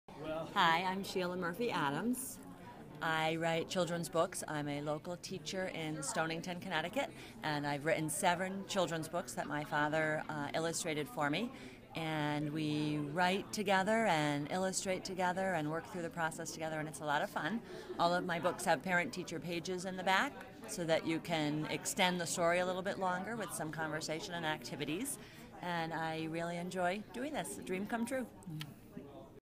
Sheila Murphy Adams - CT teacher and children's book author
Visit Sheila Murphy Adams and her books This was filmed at the "Annual Authors' Festival" at the Groton Public Library in Connecticut on November 5, 2016 by Lisa Saunders for inclusion in "The Lisa Saunders Show."